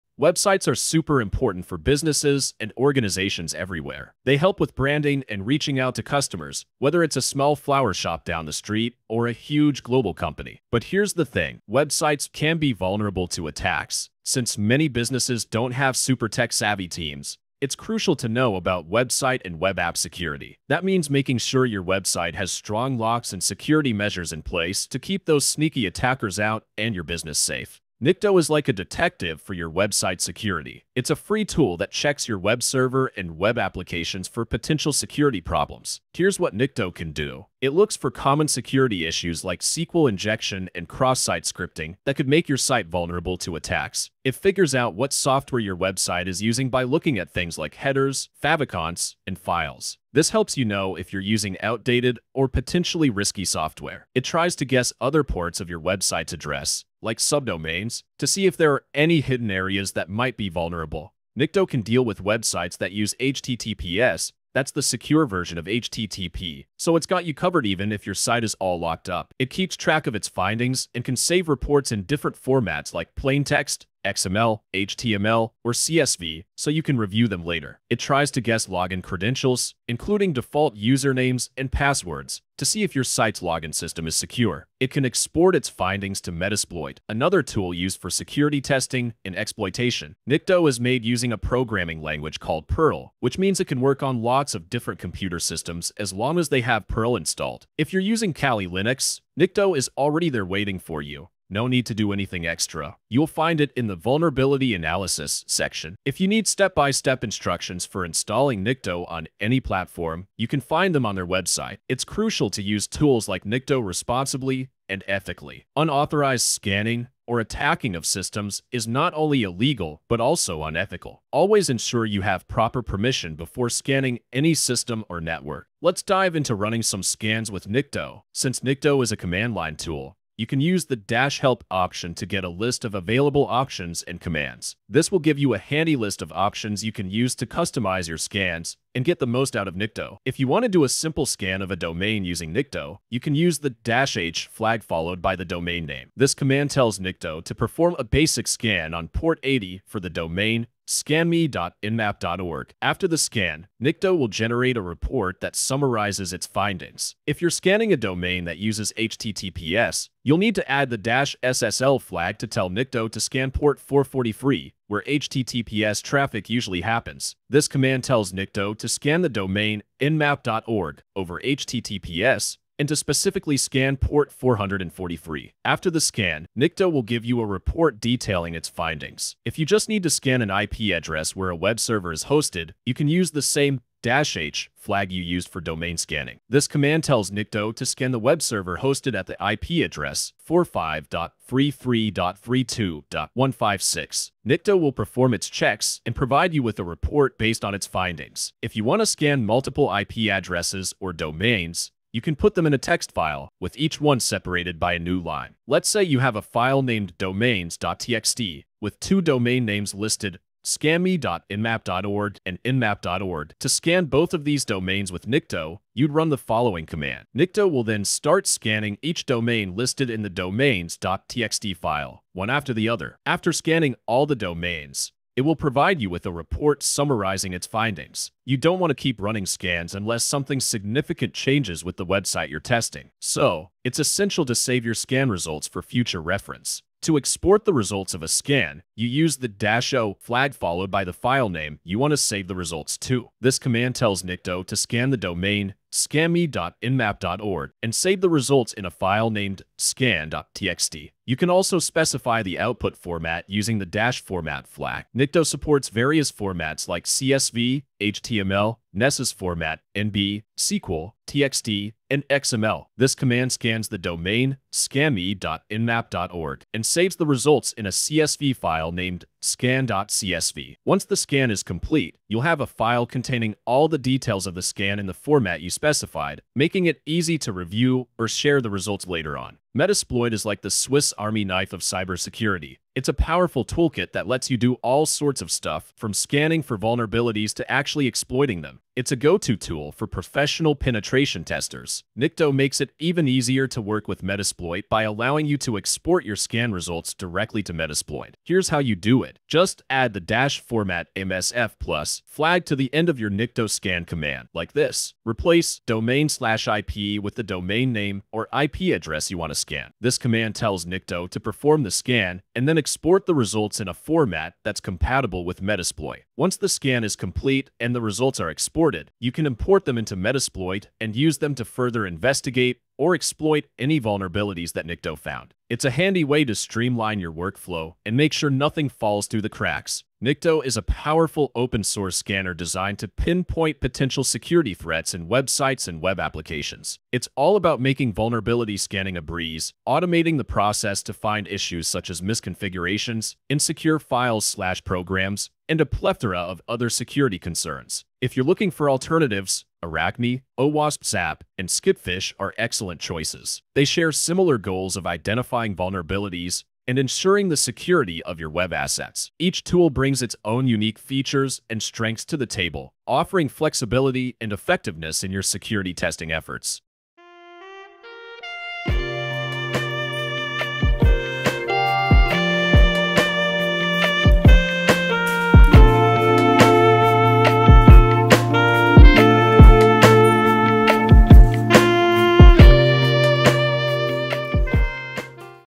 How to Scan ANY Website for Vulnerabilities!
0:00 Intro
01:30 Installation of Nikto
01:51 How to Scan with Nikto
02:19 Scan a Domain
02:36 How to Scan a Domain with SSL Enabled
02:59 How to Scan an IP Address
03:17 How to Scan Multiple IP Addresses From a Text File
03:56 How to Export Scan Results
04:39 How to Pair Nikto with Metasploit
05:47 Nikto Alternatives
05:29 Conclusion